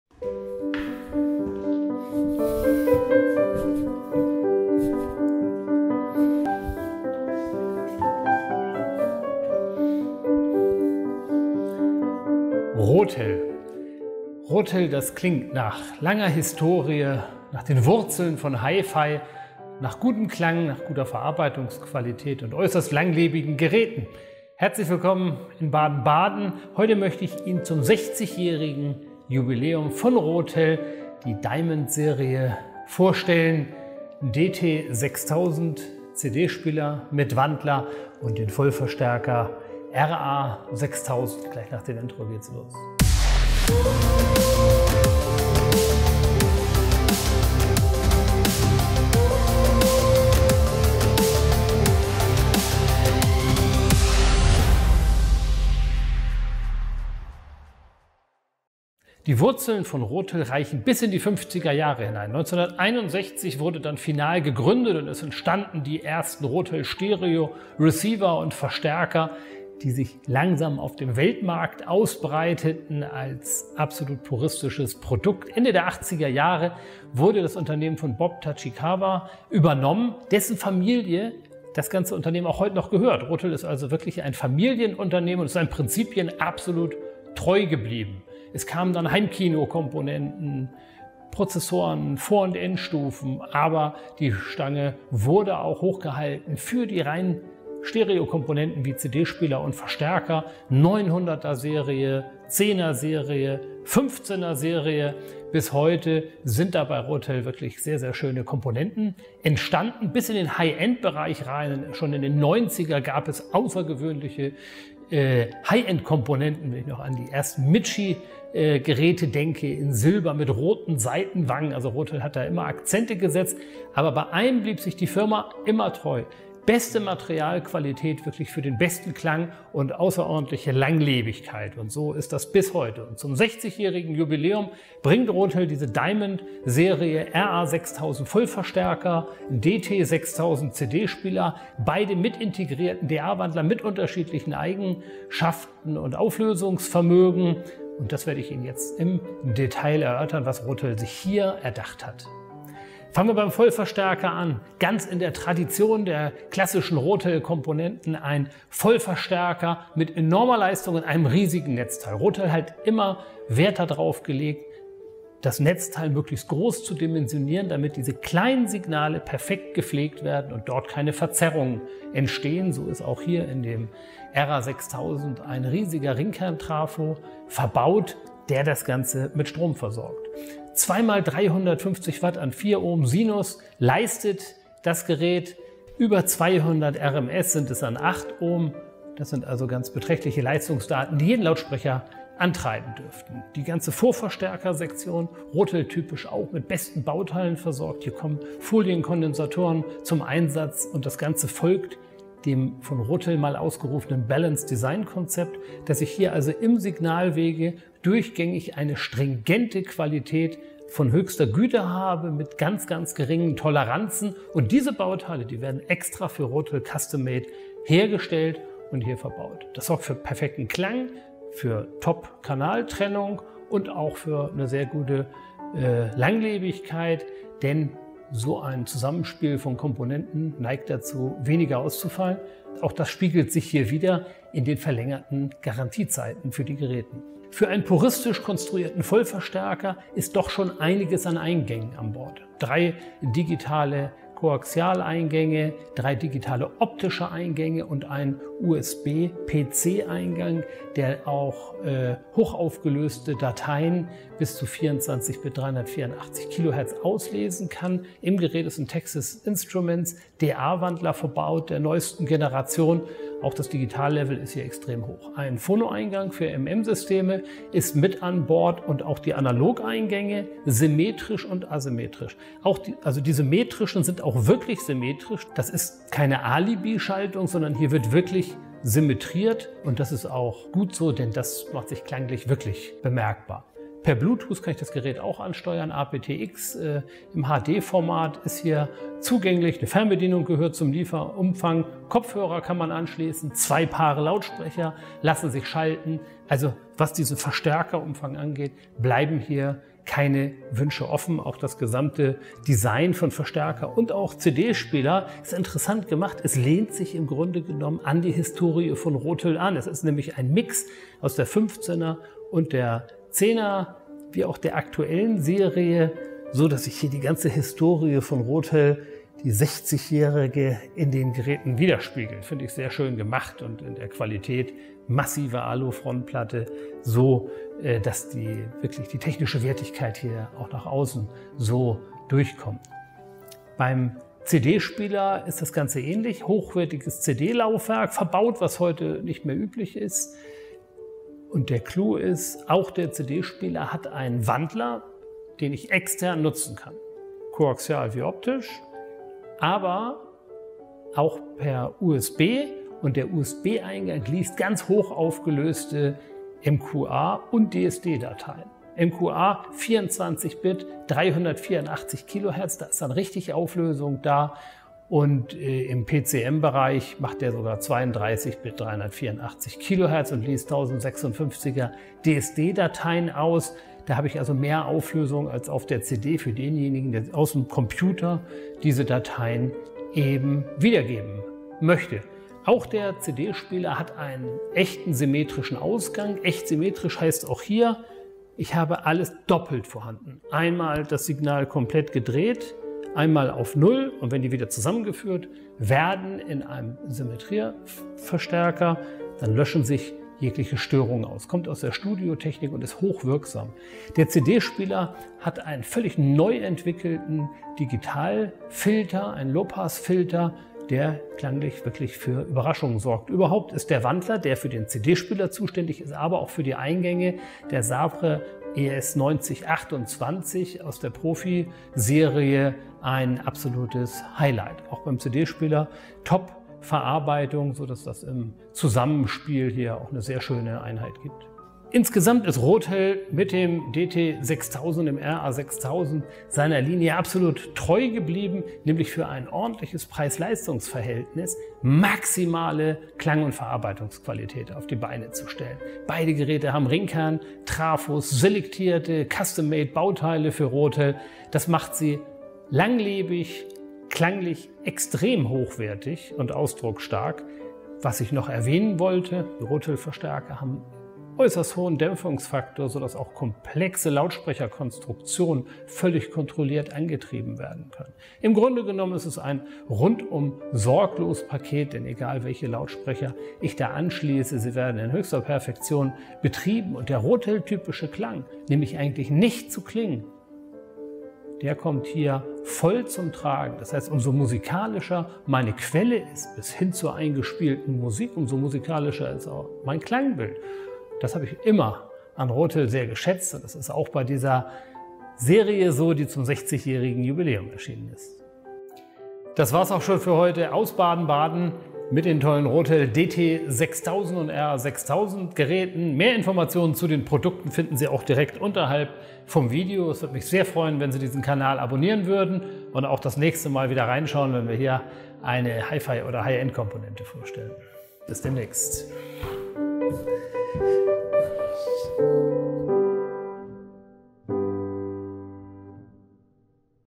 Rotel DT-6000 / RA-6000 | Diamond-Serie - 60 Jahre Rotel-Qualität!
Rotel DT-6000 und Rotel RA-6000 heißen die Geräte aus Rotels neuer Diamond-Series. Zum 60 Jährigen Firmenjubiläum bringt die bereits 1961 gegründete HiFi-Legende Rotel diese edle Kombination aus CD-Player/DAC und Vollverstärker. Dabei Rotel stand schon immer für solide, puristische Komponenten mit audiophilen Klangeigenschaften, gepaart mit großartiger Verarbeitungsqualität. Und das sieht man den neuen Geräten auch an. Das Design der Sonderserie ist ein gelungener Mix aus Moderne mit einer Formensprache, die Rotel seit vielen Jahren spricht. Da wird viel Aluminium verbaut, was der Mechanik und damit auch der Klangqualität zu gute Gute kommt. Erhältlich sind die Geräte in schwarz und silber.

Mit dem DT-6000 setzt Rotel nochmal ein klares Zeichen für den Liebhaber großer CD-Sammlungen. Der verbaute Sabre-Wandler dient aber nicht nur der CD-Wiedergabe, sondern es finden auch externe Digitalquellen Zugang - bis hin zu MQA oder DSD 1024 Formaten. Ein schaltbarer Low-Pass-Filter ist ein echtes, schon früher von Rotel eingesetztes Asset, welches klanglich einige Vorteile bringt. Ausgegeben wird das Signal digital oder analog über Cinch oder echte XLR Ausgänge.

Der Rotel RA-6000 Vollverstärker leistet satte 350 Watt Sinus an 4 Ohm pro Kanal. Ein riesiger Ringkerntrafo sorgt für die Stromversorgung. Echte XLR-Eingänge, Cinch und sogar ein Phonoeingang für MM-Systeme sind an Bord. Dazu wurde ein hochklassiger DA-Wandler eingebaut. der USB- auch MQA-Daten verarbeiten kann. Per Bluetooth kann die Musik direkt aus dem Smartphone oder Tablet abgespielt werden. Ebenso kommen Kopfhörerfans mit dem integrierten Verstärker auf ihre Kosten.

Wir wünschen Ihnen viel Spaß beim Anschauen dieses Videos. Ihr Team vom SG Akustik HiFi-Studio.